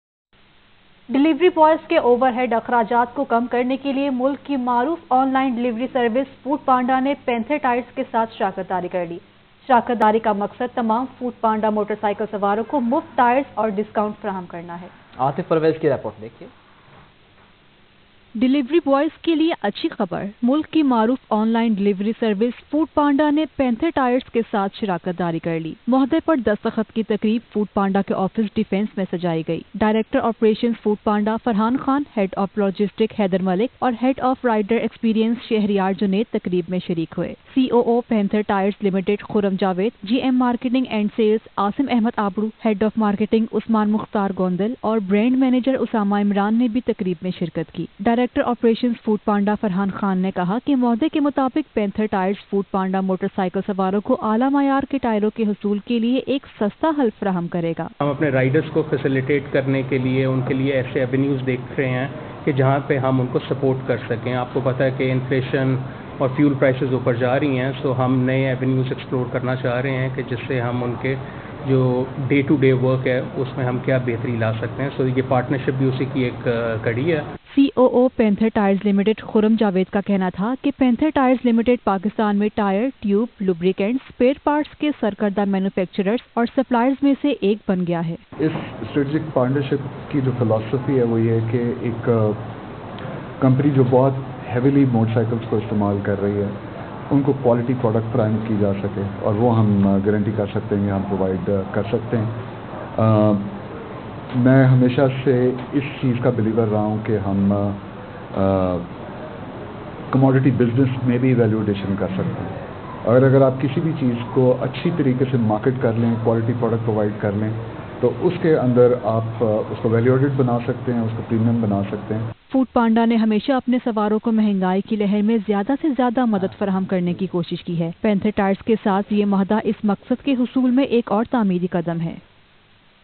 Foodpanda Partners with Panther Tyres to Facilitate Riders in Managing their Maintenance Expenses
Continuing its diverse efforts to minimize overhead costs for delivery riders, foodpanda, the country’s leading online delivery service, has partnered with Panther Tyres. The partnership aims to provide free tyres and discounts to all foodpanda riders.

A signing ceremony was held in this regard recently which was attended by Farhan Khan Director Operations, Haider Malik Head of Logistics and Sheryar Junaid, Head of Rider Experience from foodpanda’s side. While Khuram Javed COO, Asim Ahmed Abro GM Marketing & Sales, Usman Mukhtar Gondal Head of Marketing and Usama Imran, Brand Manager from Panther Tyres attended the event.

foodpanda sign agreement with Panther Tyres
Delivery riders across Pakistan have been facing the impact of rising inflation and fuel prices which ultimately cut into their disposable income. This partnership will not only make bike maintenance easy and hassle free but will also provide an opportunity to foodpanda riders to avail free-of-cost and discounts on tyres and tubes.

As per agreement Panther Tyres, will provide a cost efficient solution to foodpanda riders for acquisition of their top quality tyres. The partnership also includes after sales services at foodpanda’s hubs and pit shops.